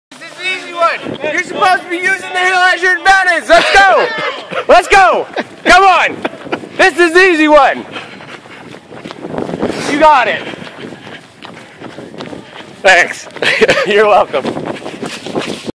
Soldier Marathon 2012 Ft Benning Georgia
Drill Sergeants, Running Downhill.